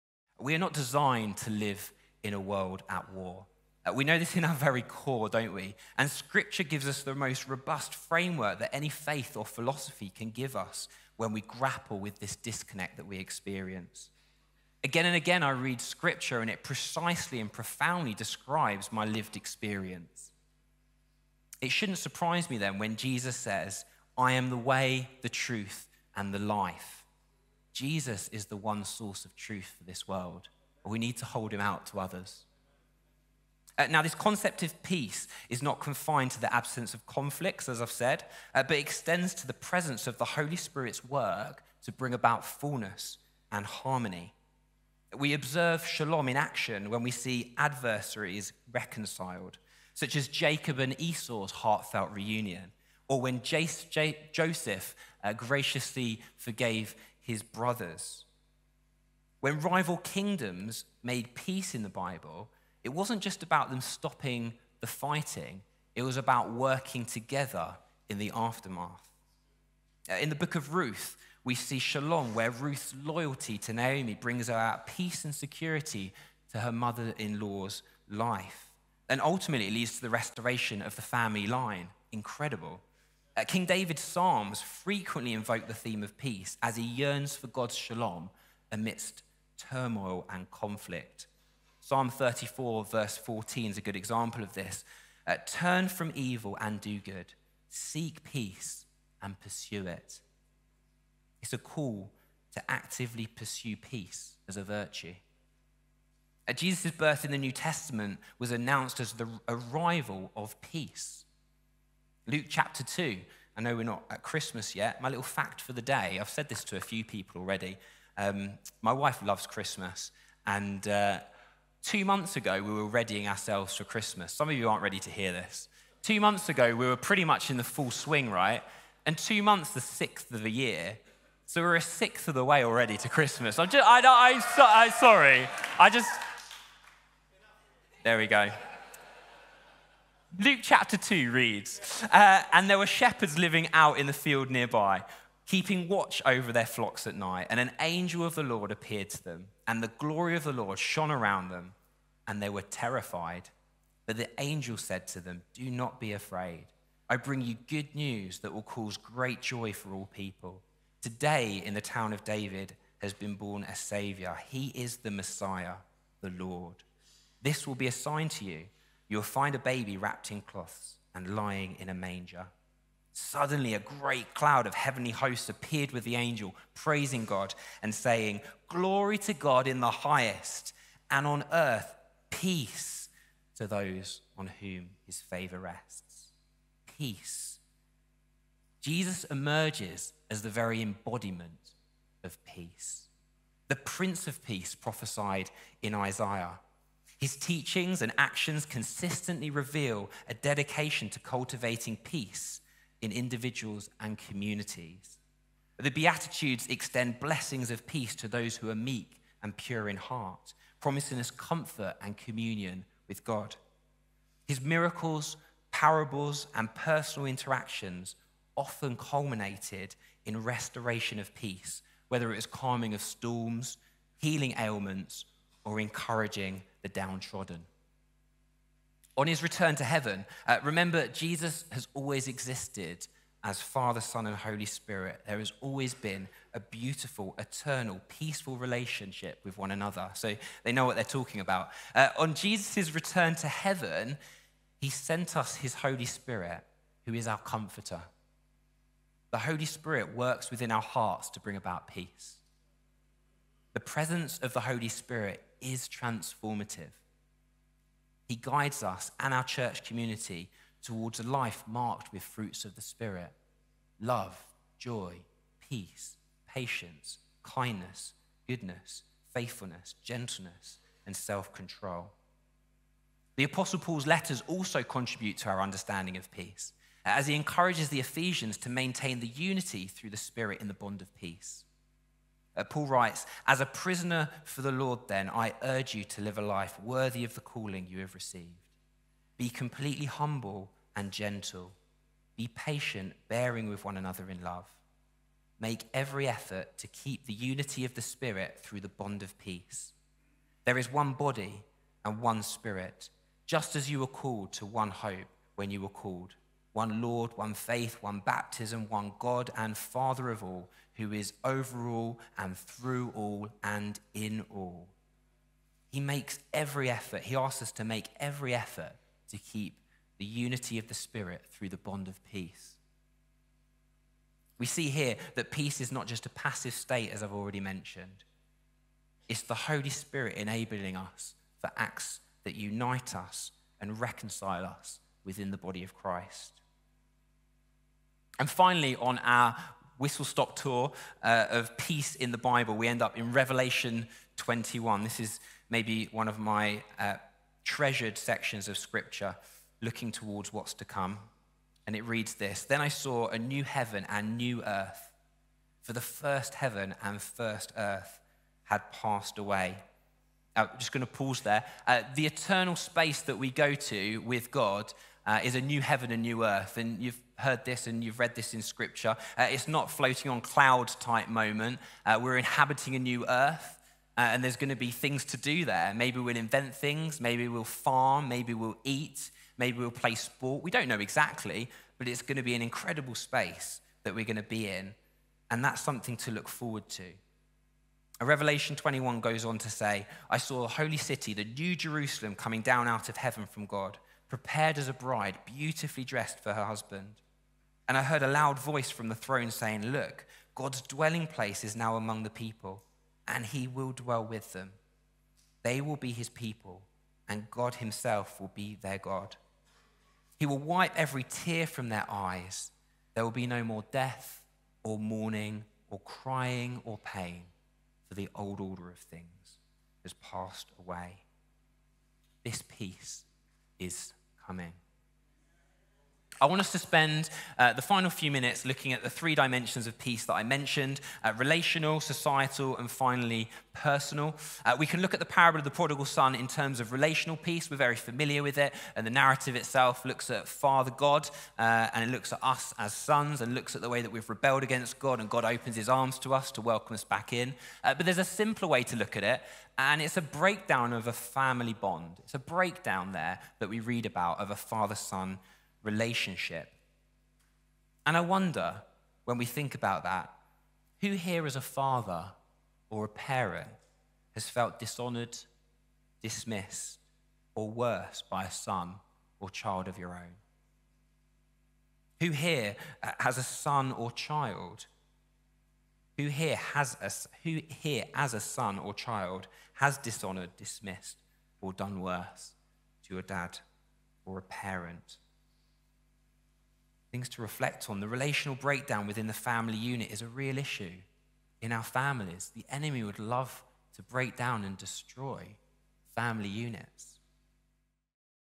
War and Peace
The core idea is that life is complex, full of moving parts and situations and relationships. And when any of these are out of alignment or missing something about shalom breaks down, life is no longer whole and it needs to be stored.